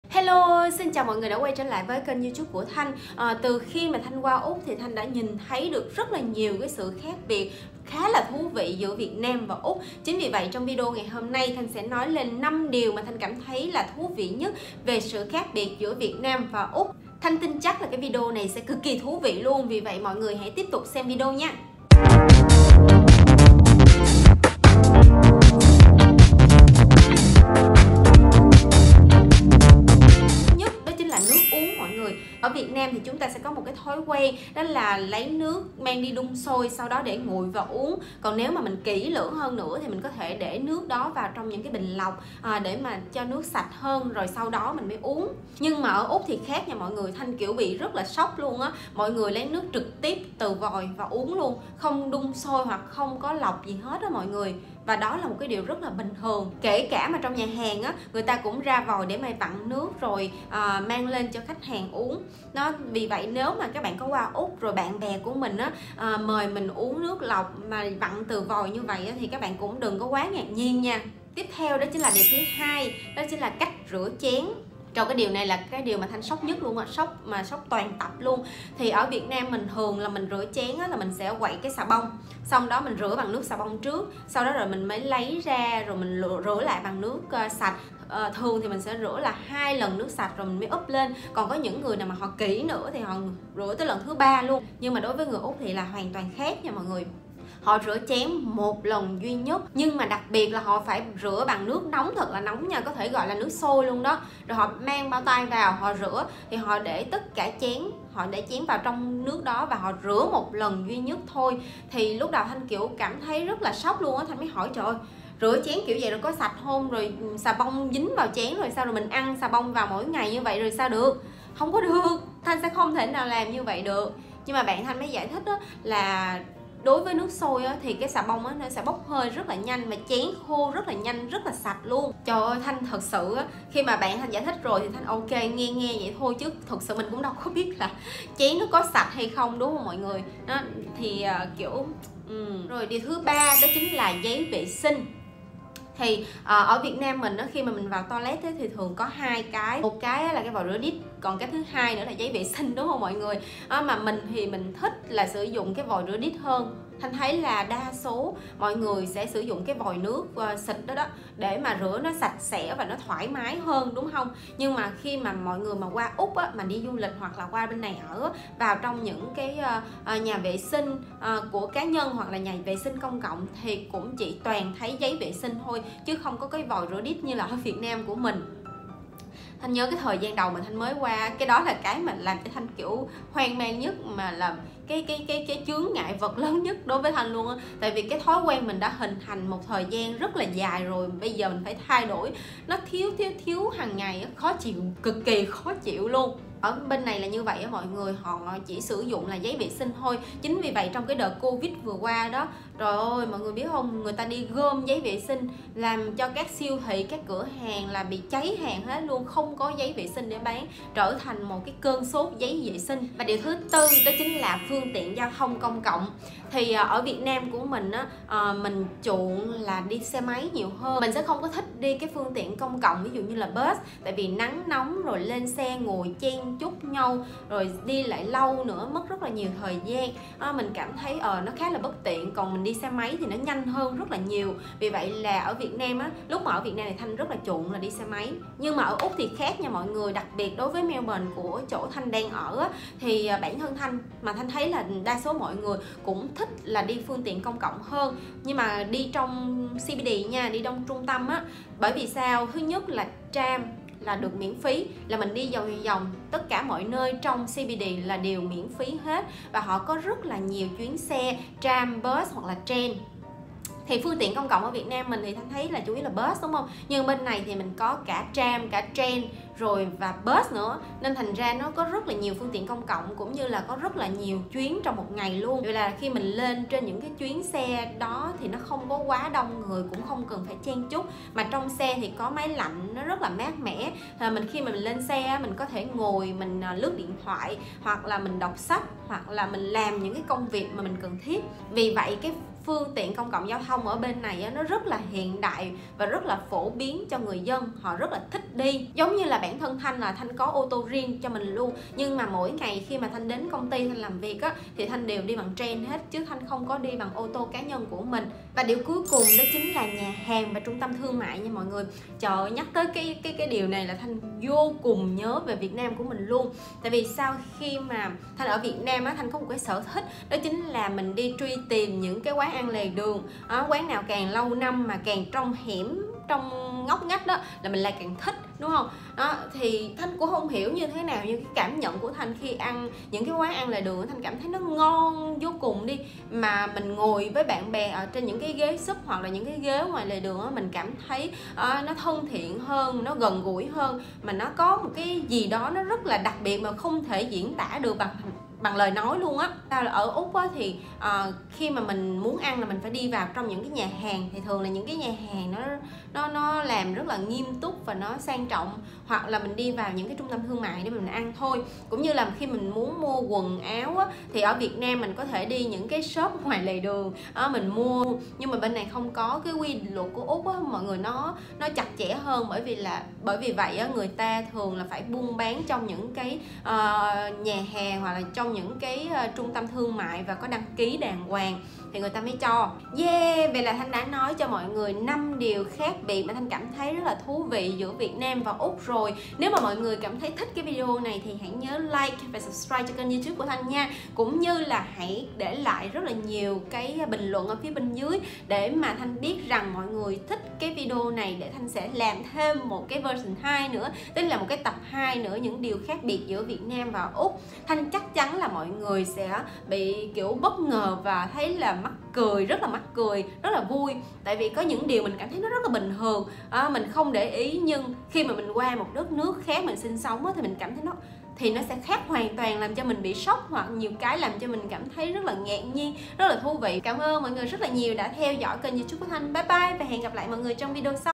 CUỘC SỐNG ÚC | 5 điều khác biệt giữa văn hóa Việt Nam và Úc
Hello mọi người, mình tên là Thanh.
Sang Melbourne sống cũng được 3 năm rồi, vì vậy trải nghiệm cuộc sống ở Úc, cũng như cảm nhận được sự khác biệt văn hóa giữa 2 nước. Trong video này, THanh chia sẻ 5 điều khác biệt giữa văn hóa Úc và Việt Nam. Hi vọng, các bạn sẽ thích video của Thanh nhé!

♡ Mình sẽ giải quyết theo yêu cầu của bạn ♡

© Bản quyền thuộc về Thanh Ngoc Au
© Copyright by Thanh Ngoc Au   ☞ Do not Reup